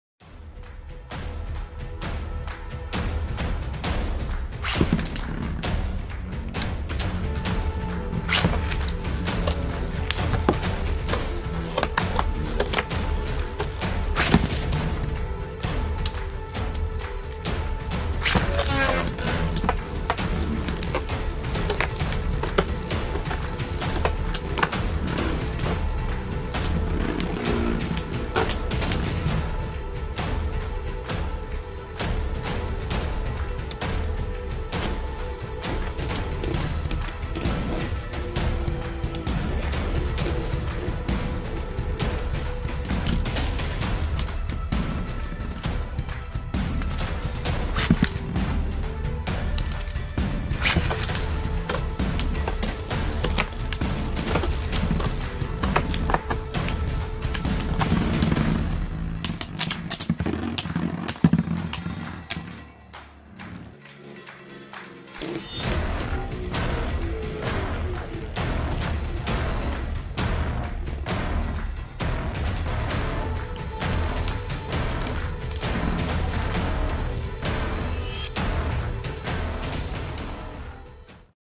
Flame Byxis 230WD vs Meteo L Drago LW105LF
The anime battle between Zeo and Ryuga !
you can still join my Limited Format CBT (2 spots left)